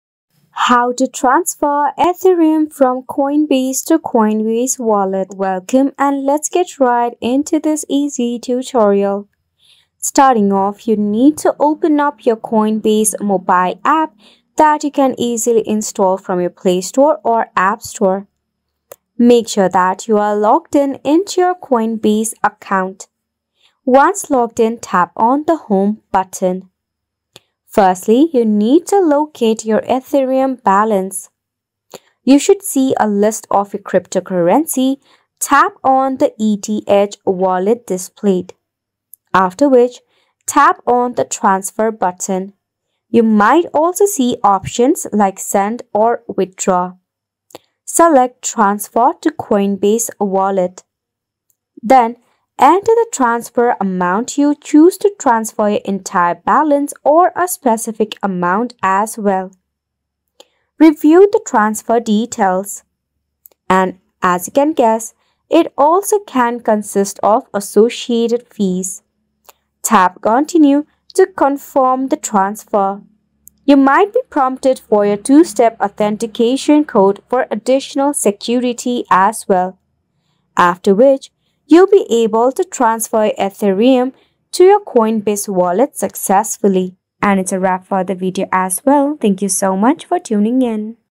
How to Transfer Ethereum From Coinbase to Coinbase Wallet | Transfer Crypto, Coinbase Wallet 2024
This guide will show you exactly how to transfer Ethereum from Coinbase to Coinbase Wallet. Our step-by-step instructions will make the process smooth and simple, whether you're a seasoned trader or a beginner.

In this video, you’ll learn:
- How to set up your Coinbase Wallet
- Linking your Coinbase account to your Coinbase Wallet
- Transferring Ethereum securely and efficiently
- Tips for managing your Ethereum in Coinbase Wallet

Coinbase Wallet offers a seamless and secure way to store and manage your Ethereum. Our guide covers everything you need to know to ensure a safe and successful transfer of your crypto assets.

Make sure you stay till the end for some pro tips on managing your Ethereum and avoiding common pitfalls.